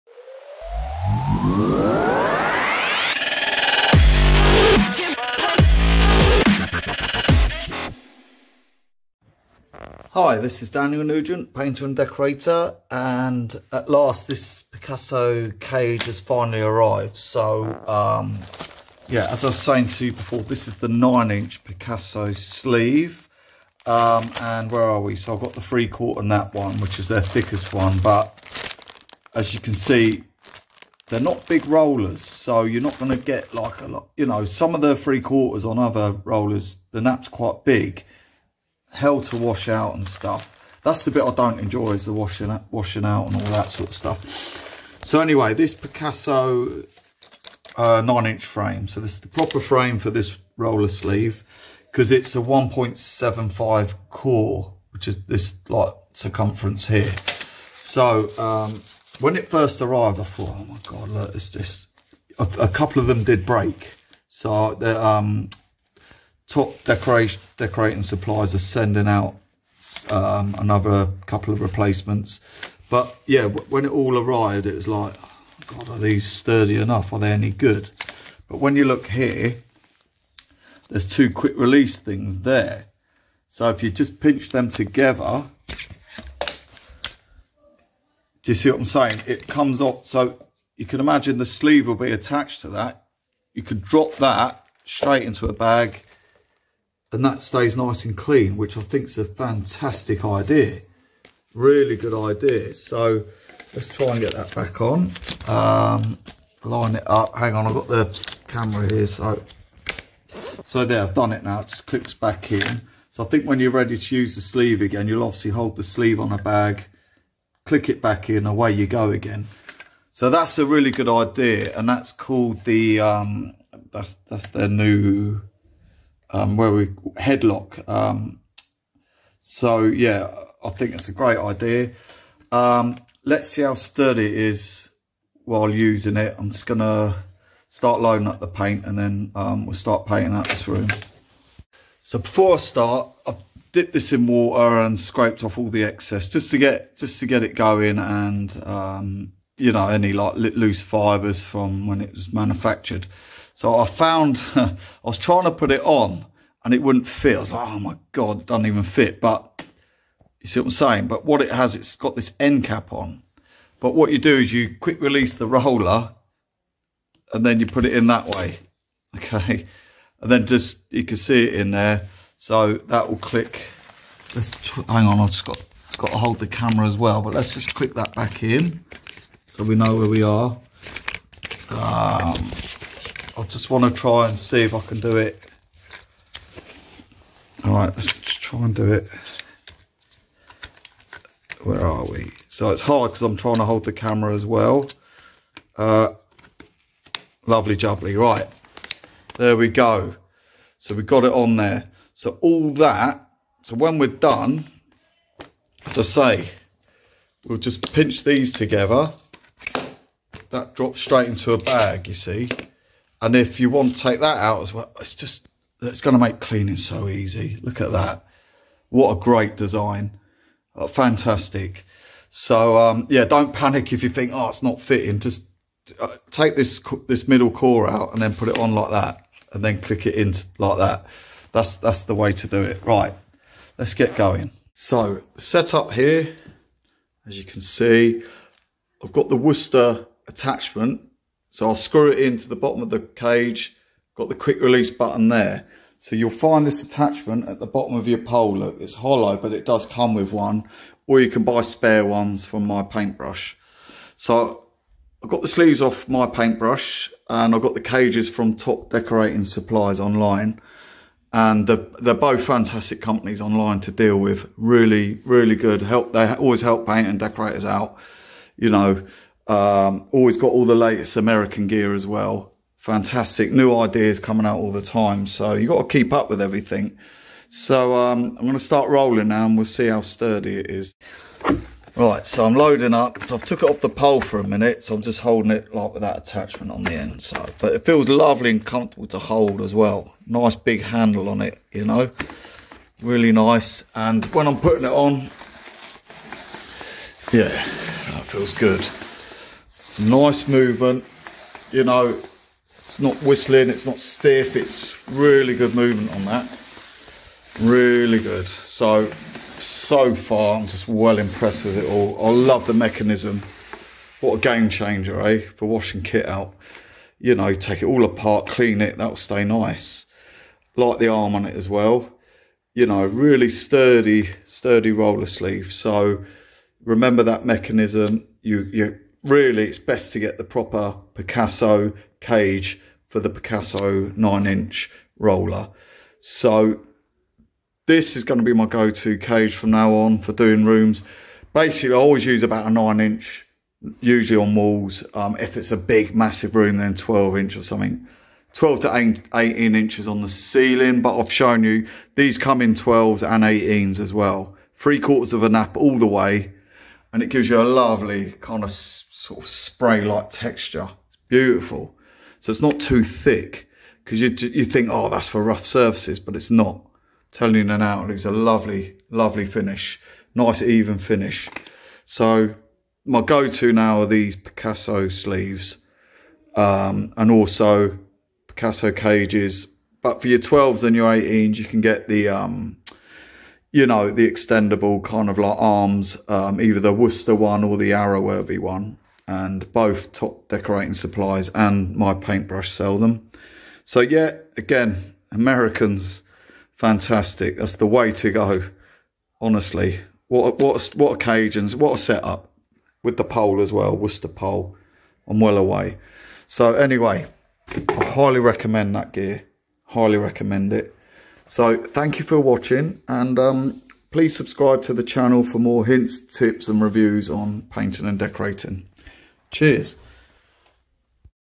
The Best Roller Frame And Sleeve Yet
I have to say what a fantastic design. it's like nothing before. A whole new way of taking a sleeve on and off with ease.
For me this is a game changer and used with the Wooster Sherlock Pole and Conversion Tip so it can also be quick release too.
aswell as the other company. I switch between them due to stock so give them a try. My new goto rollers and cages.